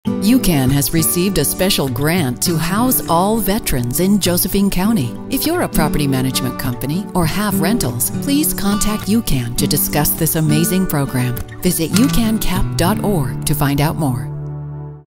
UCAN Veterans Housing :15
UCAN has received a special grant to house all veterans in Josephine County. If you’re a property management company or have rentals please contact UCAN to discuss this amazing program! Visit ucancap dot org to find out more!